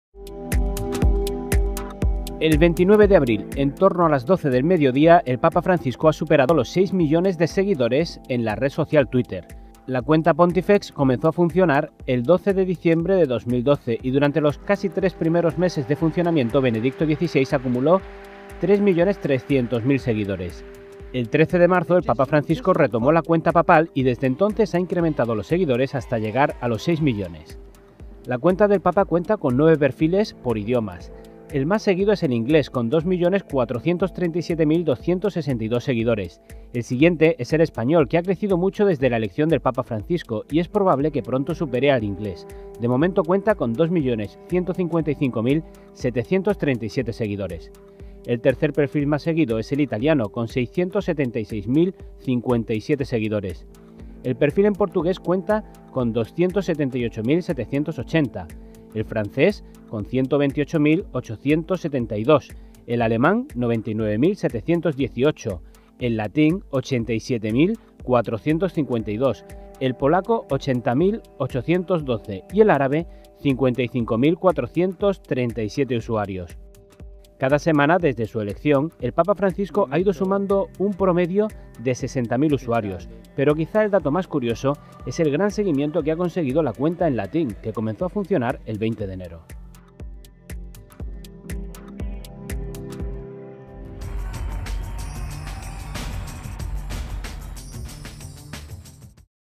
El papa Francisco alcanza los 6 millones de seguidores en Twitter
El 29 de abril, en torno a las 12 del mediodía, el papa Francisco ha superado los 6 millones de seguidores en la red social Twitter.

La cuenta Pontifex comenzó a funcionar el 12 de diciembre de 2012 y durante esos casi tres meses de funcionamiento Benedicto XVI acumuló 3,3 millones de seguidores.